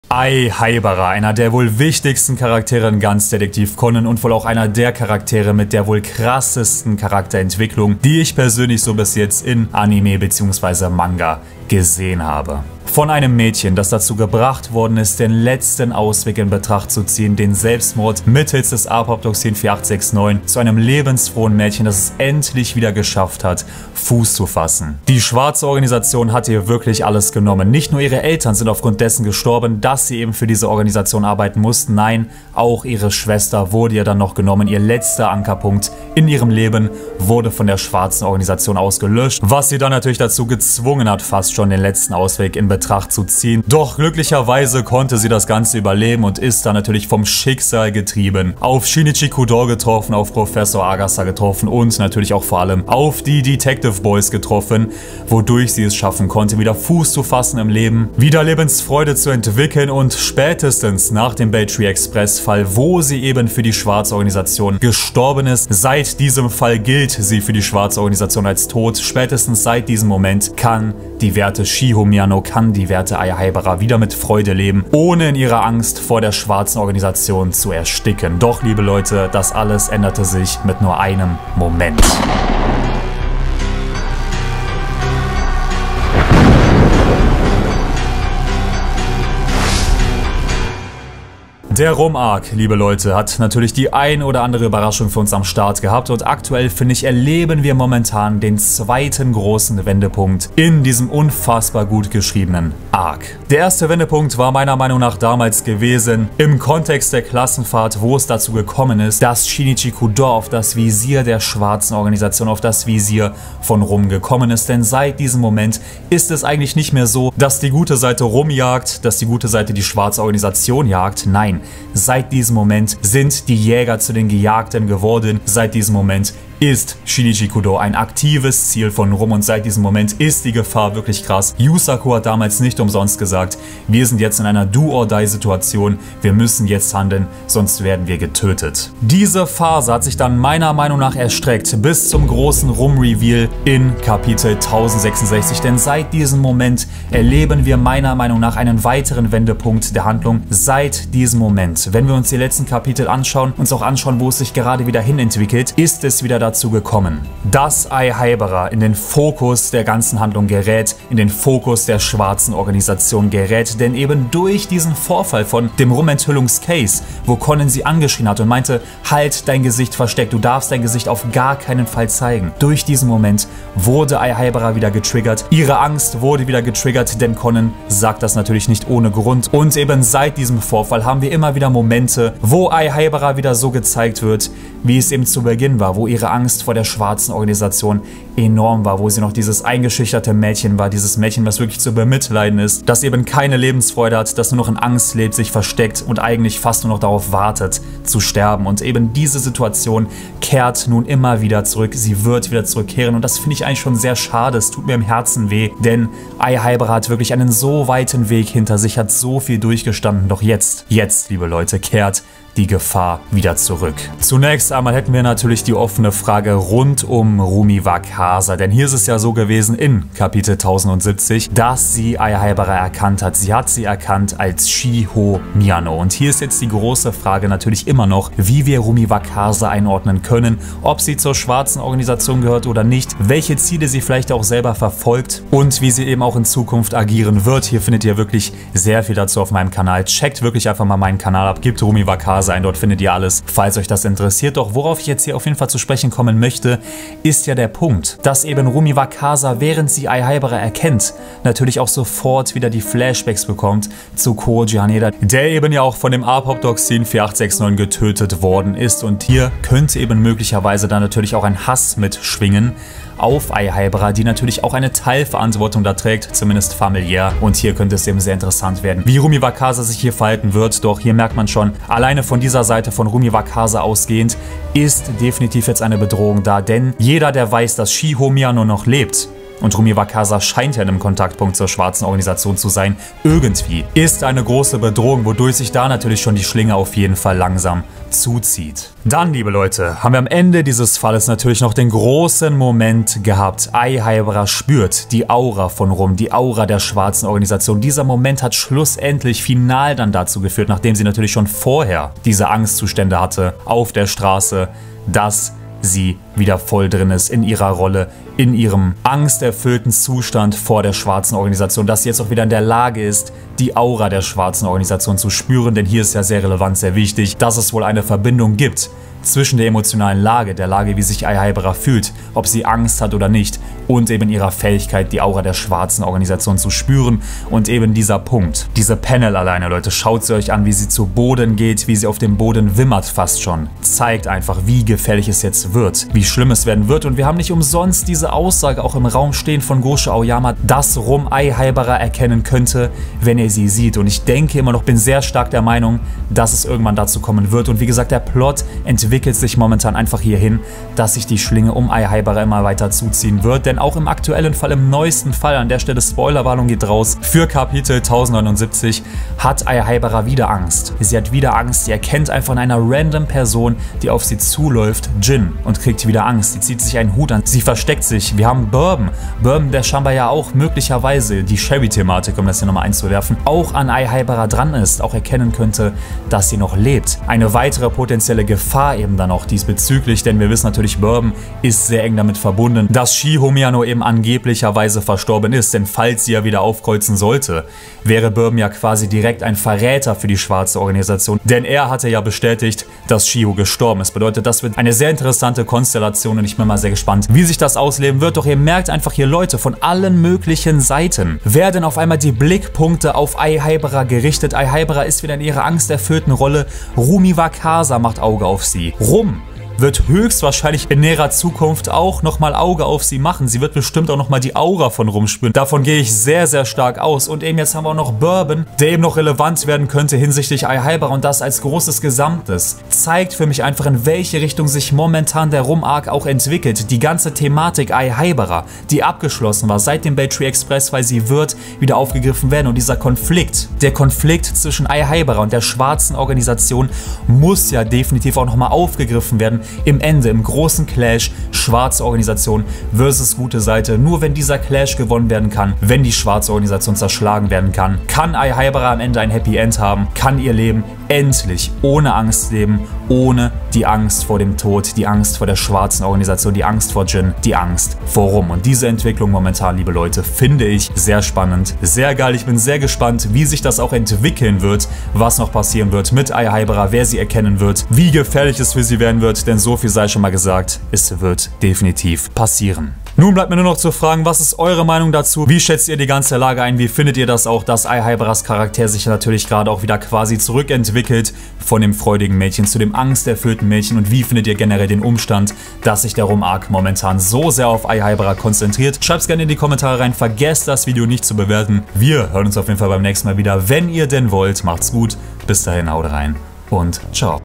Der WENDEPUNKT im RUM ARC!?💥 Die GEFAHR kehrt zurück!! // DETEKTIV CONAN
Seit einigen Kapiteln konzentriert sich die Handlung mit einem sehr großen Augenmerk auf Ai Haibara, die in ihren Angstanfällen wieder verstrickt ist und von allen Seiten belauert wird!

Wichtiger Hinweis!
• Alle Rechte der gezeigten Bilder und der Musik gehören nicht mir!